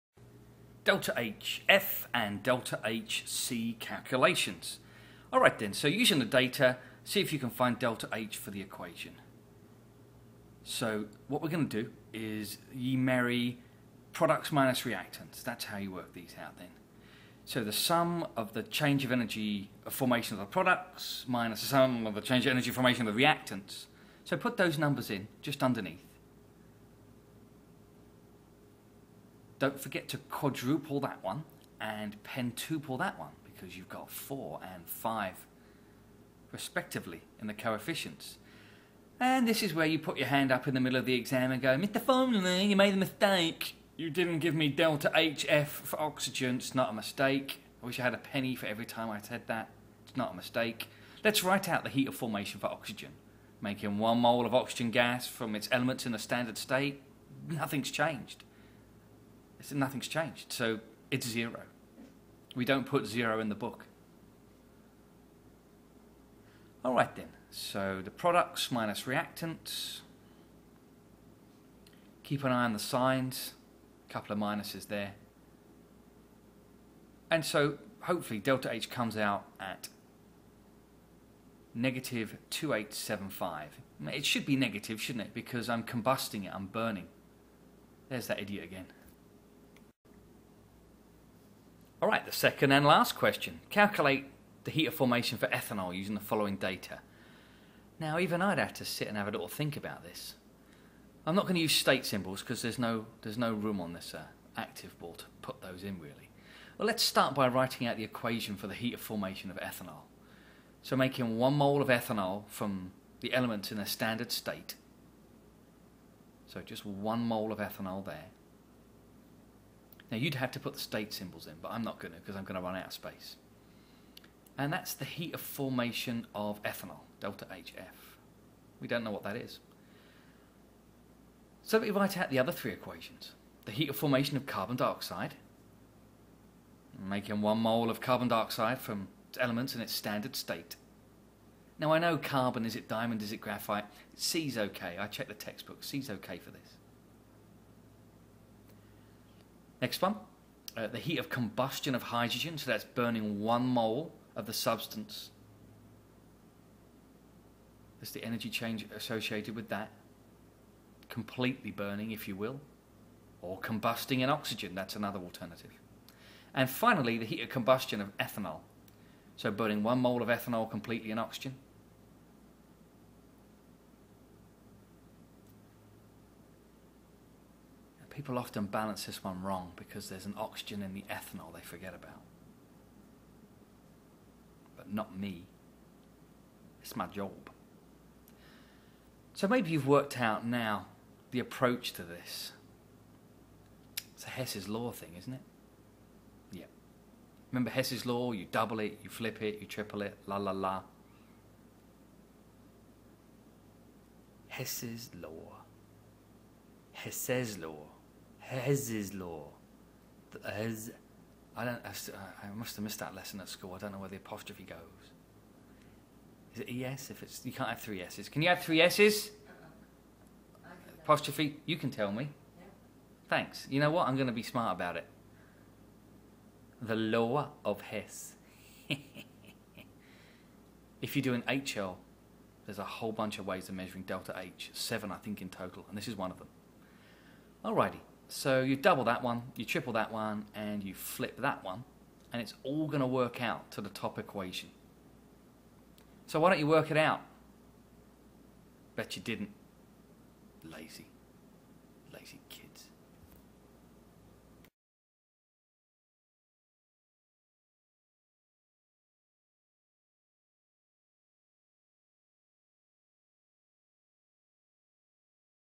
R1.2.4 Delta Hf calculations [HL IB CHEMISTRY]
Determine the enthalpy change of a reaction using standard enthalpy changes of formation and combustion. This is the old products-reactants business or maybe a cheeky Hesssssssssesssse'ssses''s'''' Law.